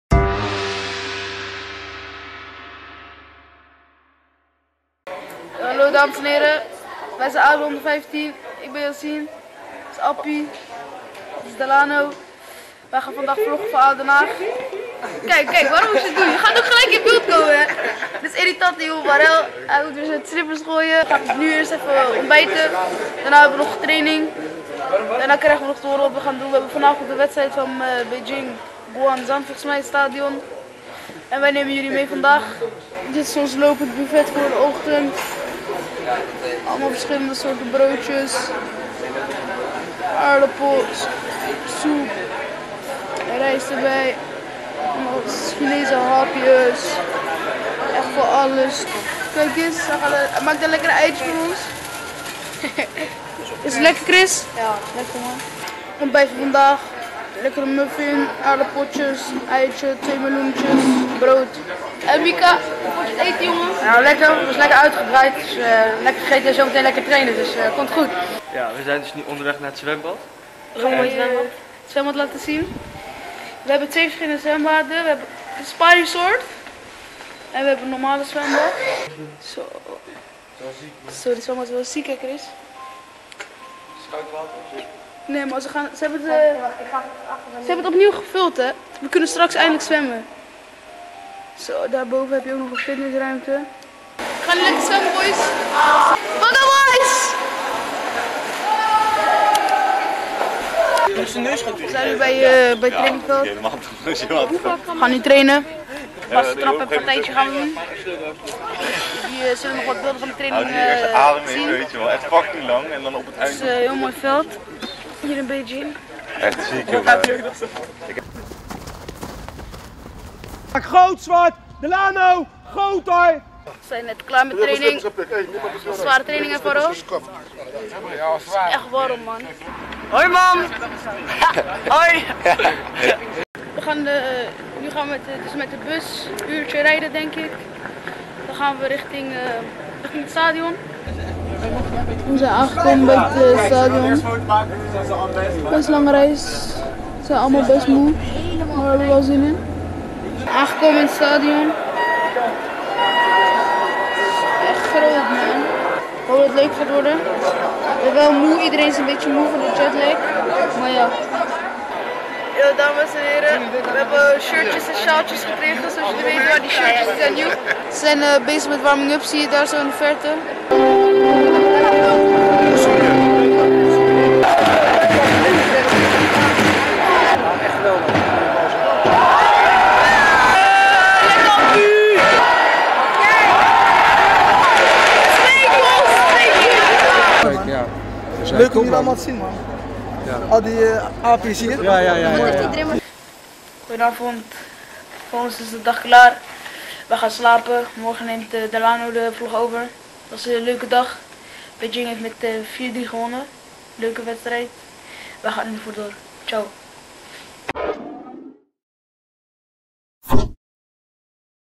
ADO Den Haag onder 15 in China: VLOG #1
ADO Den Haag onder 15 verbleef van 27 april t/m 5 mei in China. In drie vlogs doen de jongens verslag van deze geweldige reis!
Vandaag deel 1 door Yassine Ben Salah!